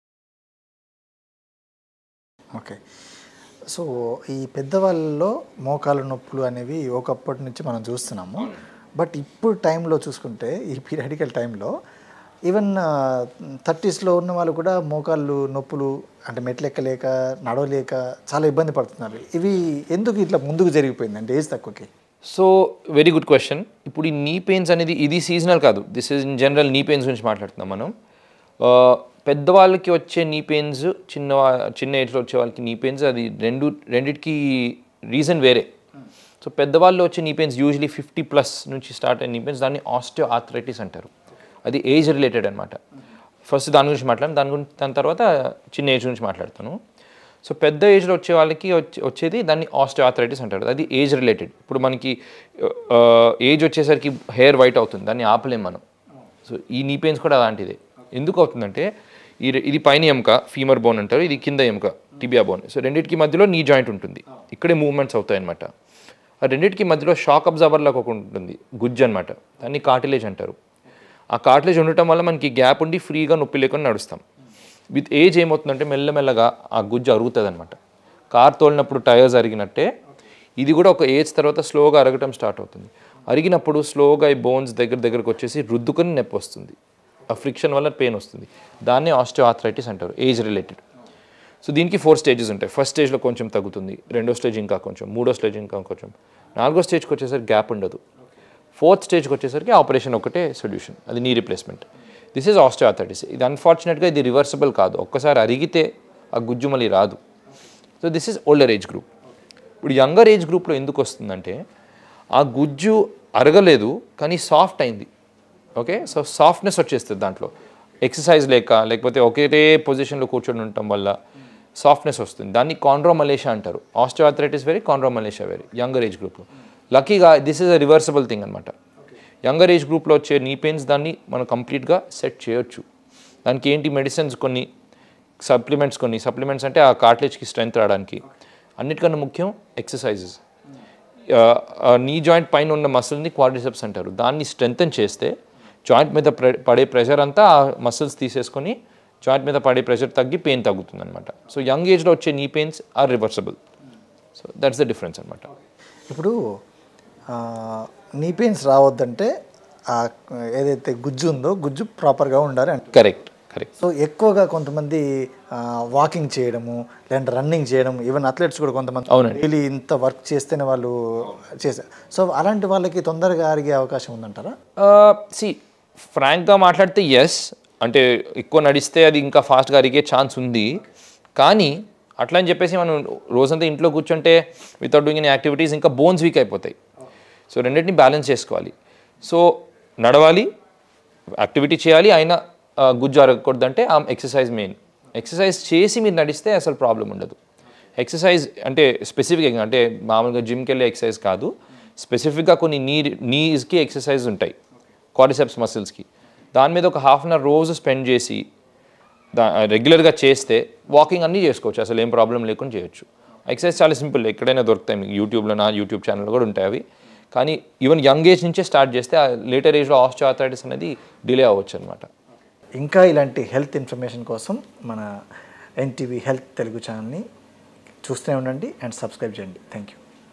అందుకే.. 30 ఏళ్లకే మోకాళ్ళ నొప్పులు! | Dr Adarsh Annapareddy | Orthopedics | NTV Health Telugu
That's why.. Facing Early Knee Pains! | Dr Adarsh Annapareddy | Orthopedics | NTV Health Telugu

NTv Telugu News Channel Presents Ntv Health Telugu Channel.